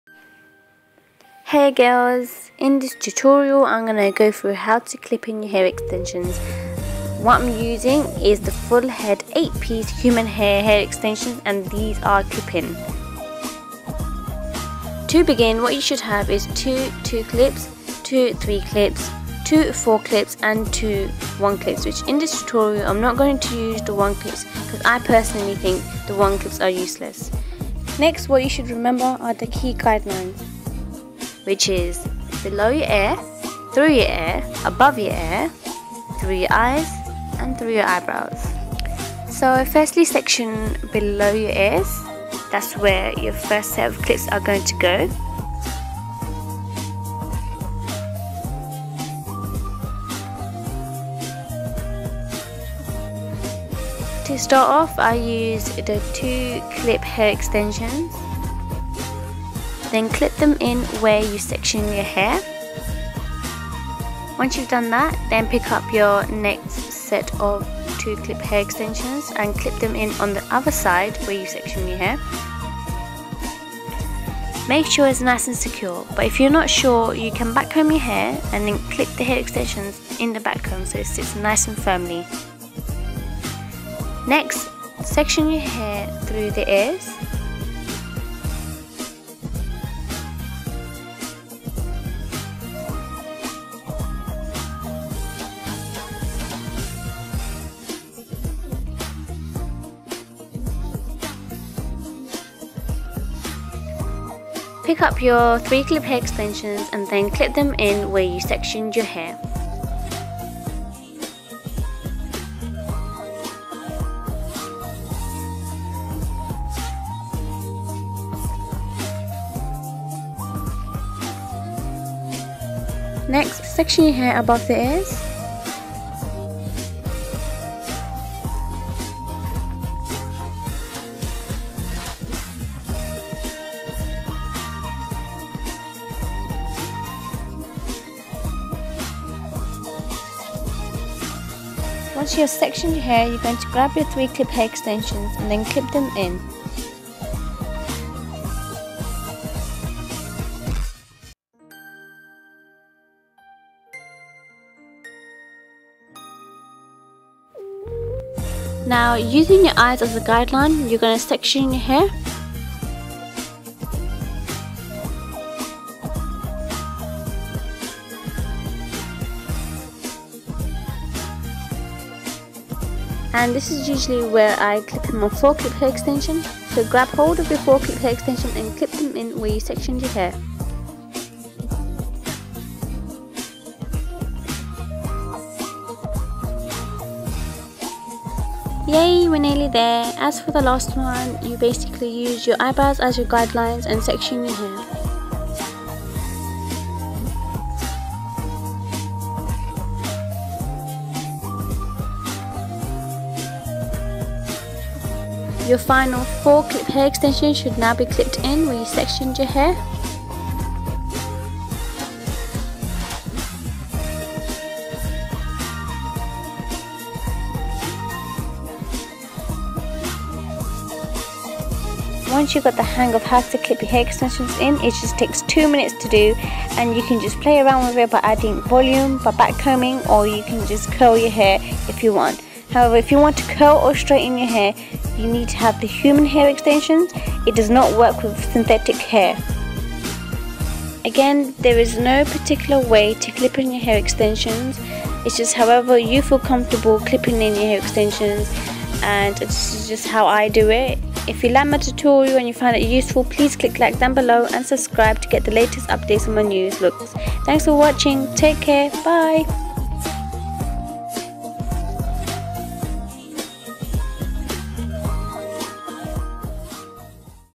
How to: Clip in Hair Extensions
Hiya!

I go through a step by step tutorial on how I clip in my hair extensions, there is no correct way to do this, you can clip them in where you feel the clips go best but this is just how I like to do it. I feel like it blends well like this on my hair.

I didn't use the 'one' clips because I didn't need them but for those who feel like the extensions still need blending in some areas you can clip them in there. You need to use a shade that matches you hair the closest so it doesn't make it obvious that you are wearing these.

I'm wearing the 'full head 8 clip in human hair extensions'